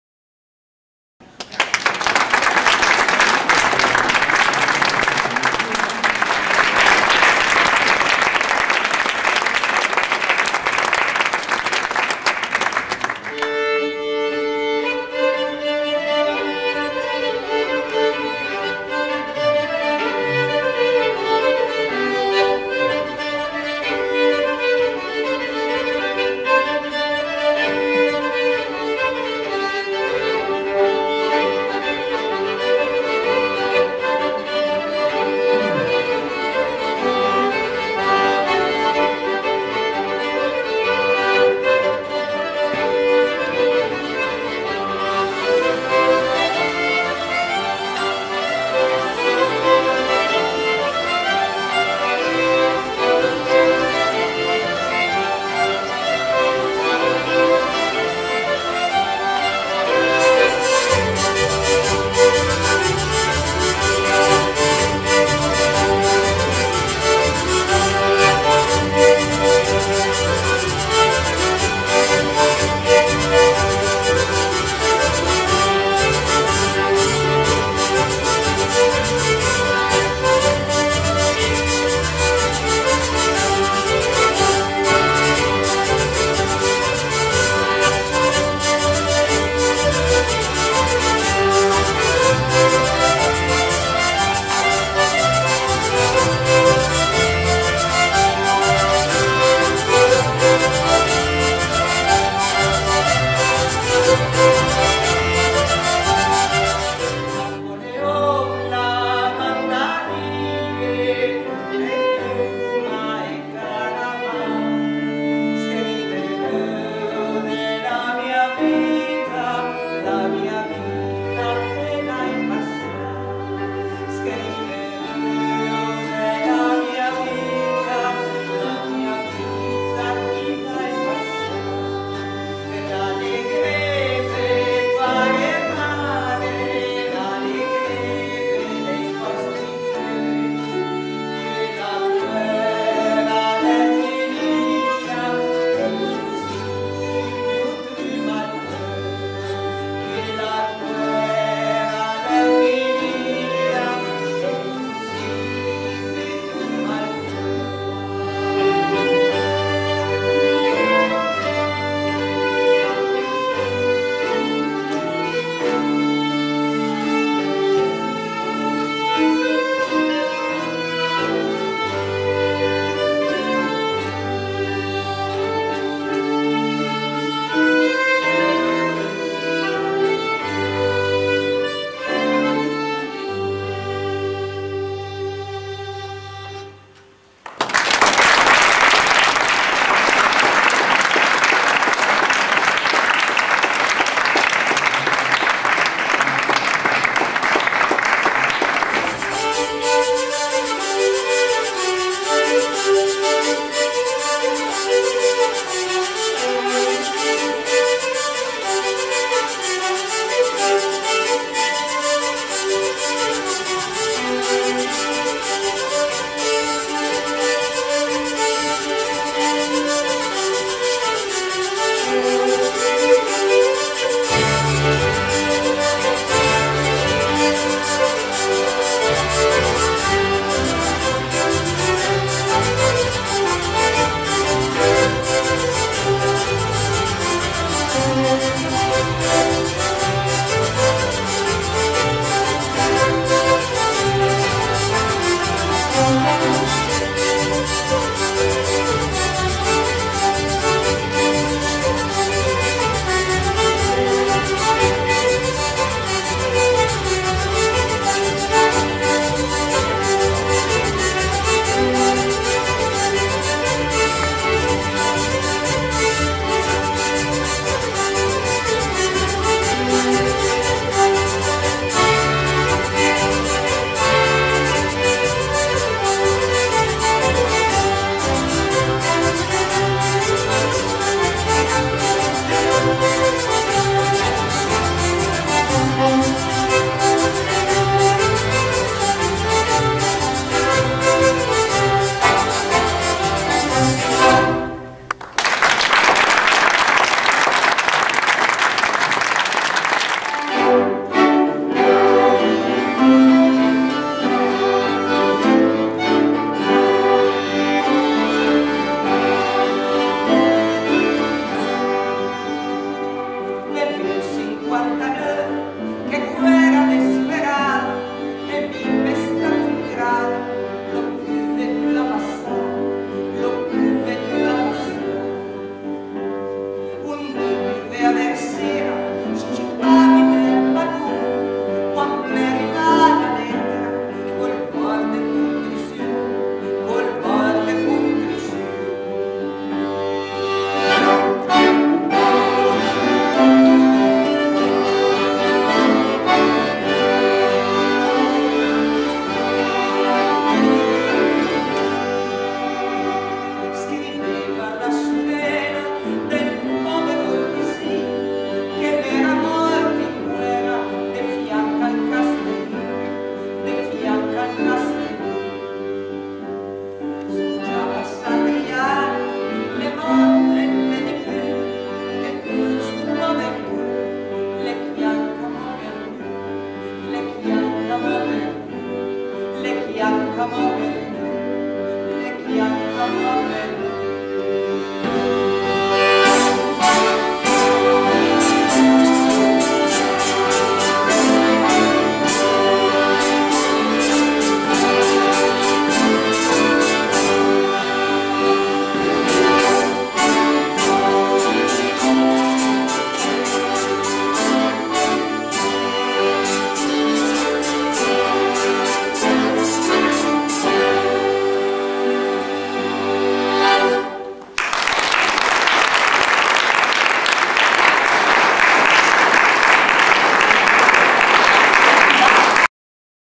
Tre Martelli al teatro del Castello di Rivoli (TO)
Estratti dal concerto dei Tre Martelli al Teatro del Castello di Rivoli (TO)
150° Unità d'Italia - Concerto confronto -19 marzo 2011
nell'ambito della stagione concertistica 2010/2011